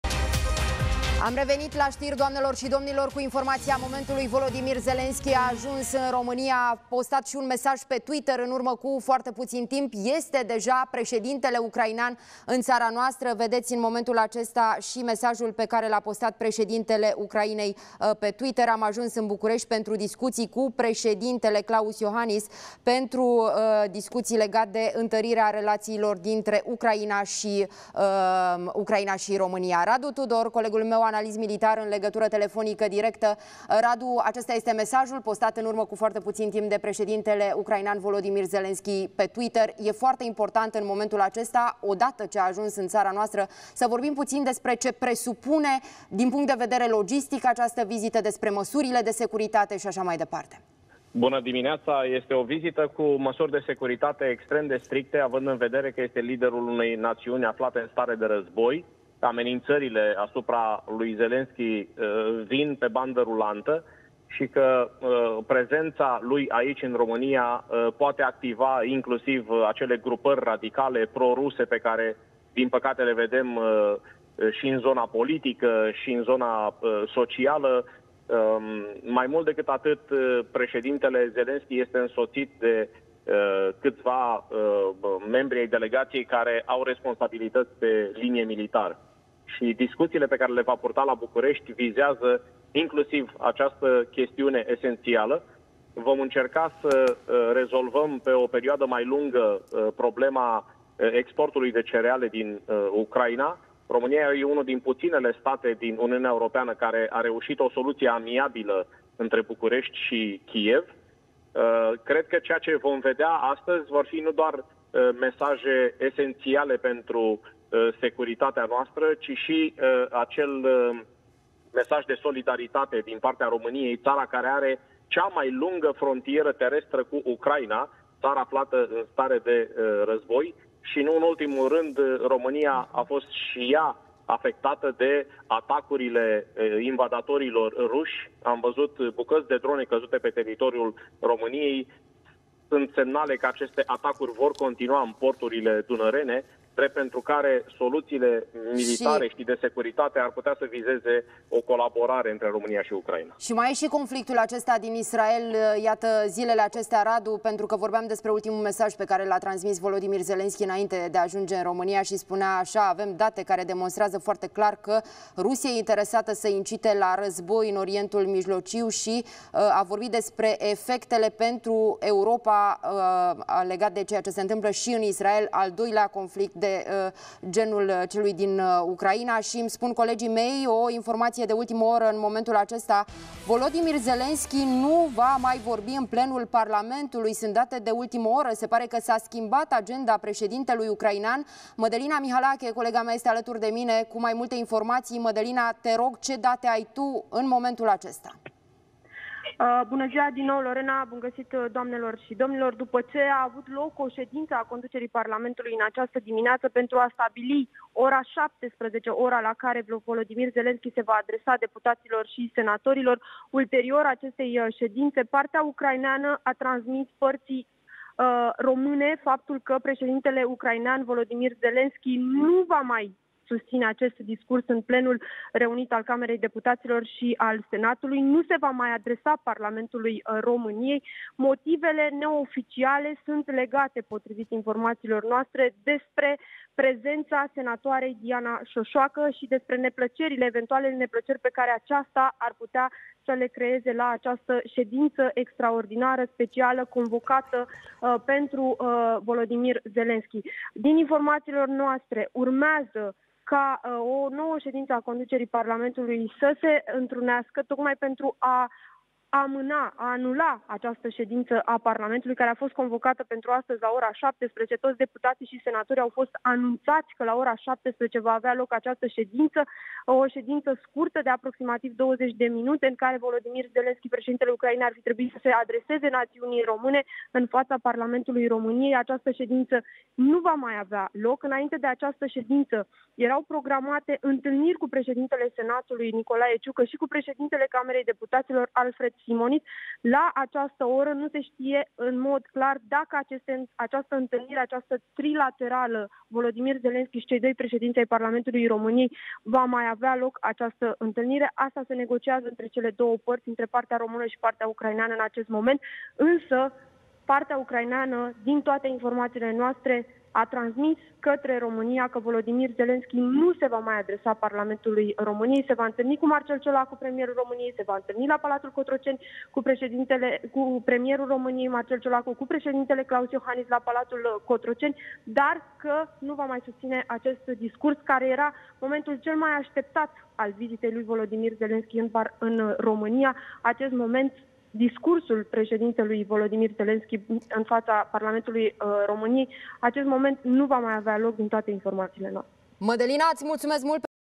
Volodimir Zelenski anunţă că a sosit în România. Schimbare de ultim moment
Discursul lui Volodimir Zelenski în plenul Parlamentului nu va mai avea loc, a anunţat partea ucraineană.